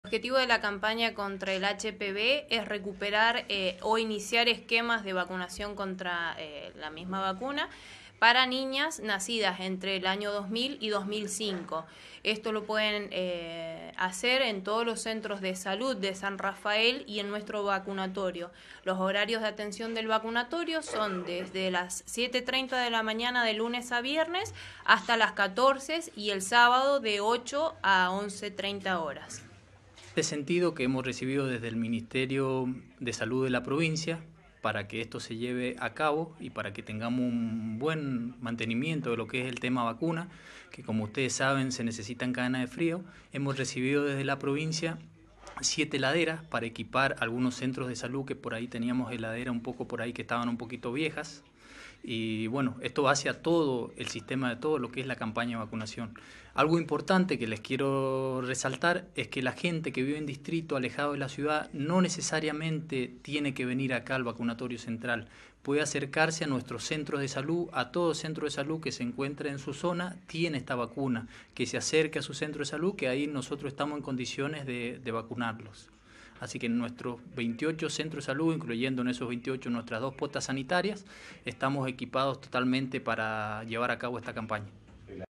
VACUNATORIOS EN TODO SAN RAFAEL
El Departamento de San Rafael Mendoza preparado para el operativo de vacunación contra el Virus del Papiloma Humano